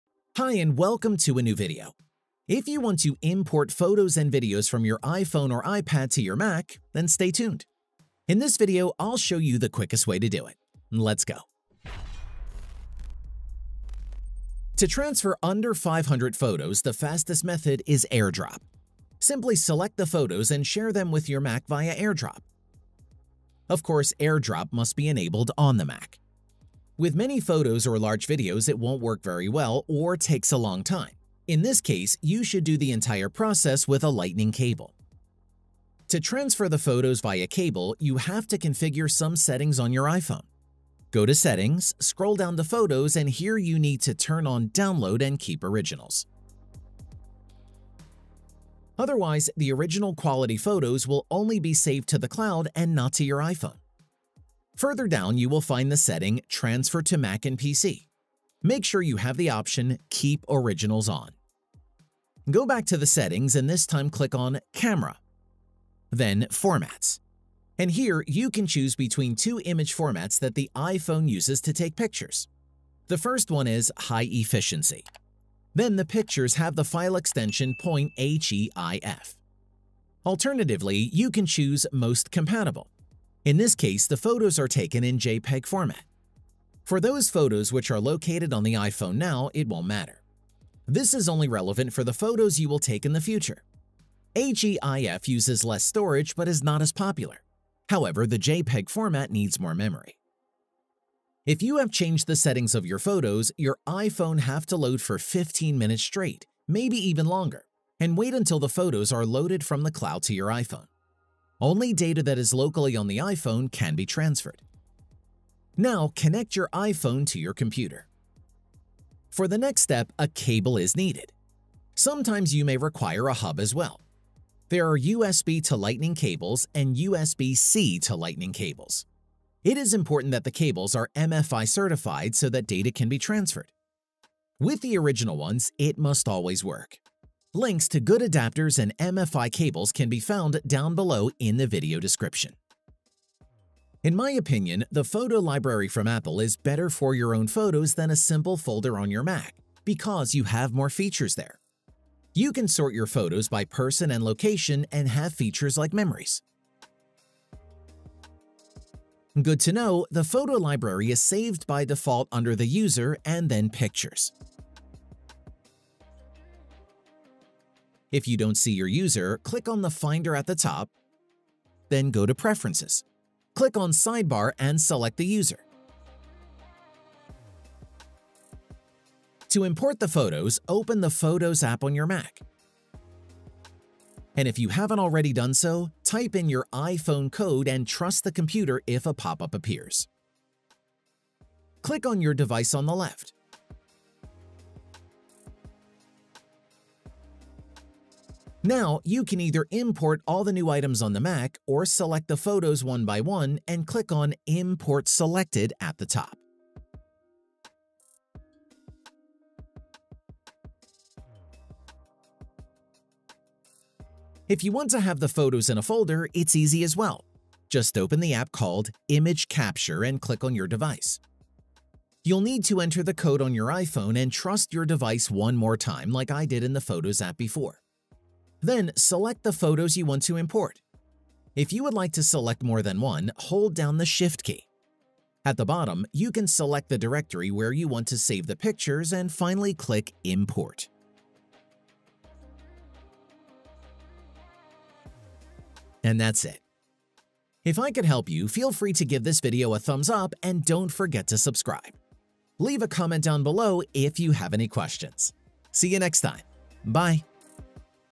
Transfer Pictures and Videos from iPhone to Mac! [2023]
Hello, in this video I'll show you how to transfer your pictures and videos from your iPhone to your Mac easily via cable, when the memory of your iPhone is full. Your Jigxor

Timestamps:
00:00 | Whom is this method suitable for?
00:36 | Necessary settings on iPhone & iPad
01:55 | Selecting the right cable
02:23 | Transfer pictures & videos with Photos app to Mac
03:34 | Save pictures & videos as files on Mac